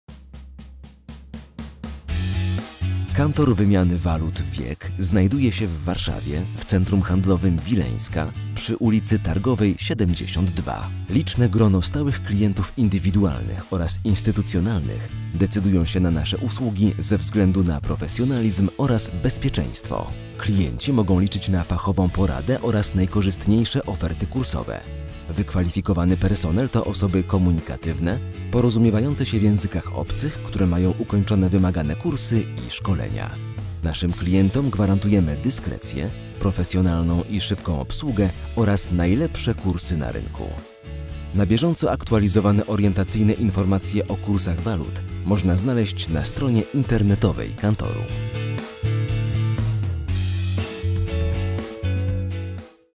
Wiek II. Kantor wymiany walut CH Warszawa Wileńska
Kantor Wiek II oferuje usługi z zakresu wymiany walut.
Nasza specjalność to wymiana pieniędzy (Exchange) i posiadamy największy wybór walut z całego świata.

Najbardziej popularne: dolar, frank, rubel, marki, funt czy euro oraz mniej znane waluty sprzedajemy i skupujemy, analizując bardziej szczegółowo niż inne kantory bieżące kursy walut.